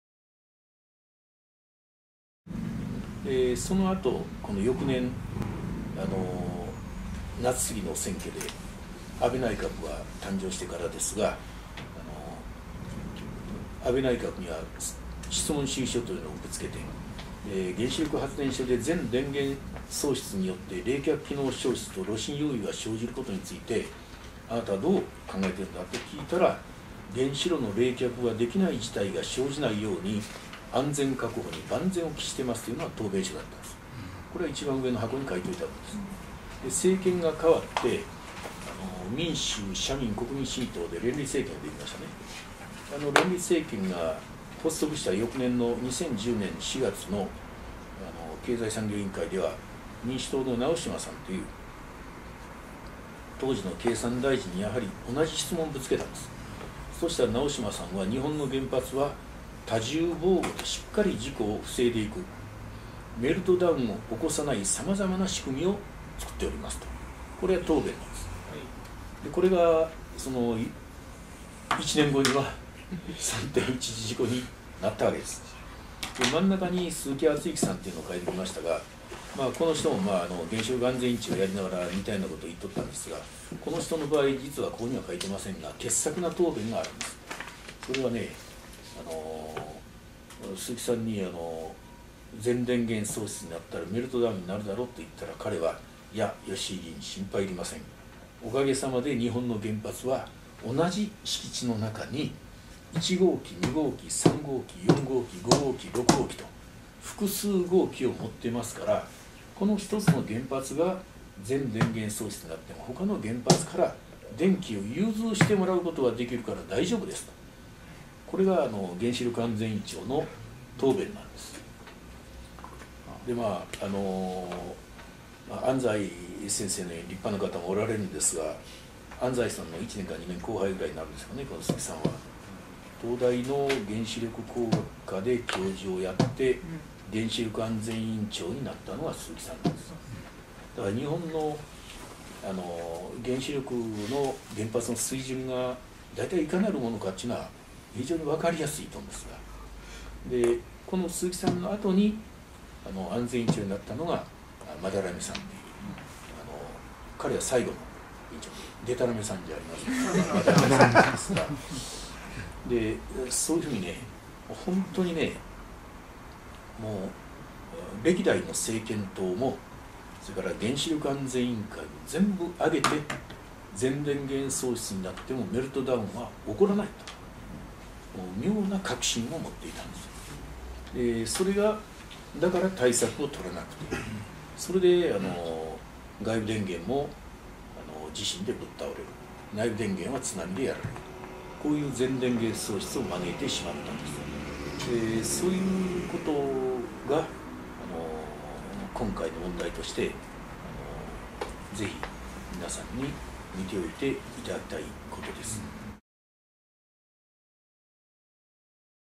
④技術史と資本の論理から読み解く『福島原発事故』by吉井英勝④全電源喪失問題の質疑
JSA大阪　哲学研究会　11月例会　2013年11月19日
吉井英勝　元衆議院議員、日本科学者会議会員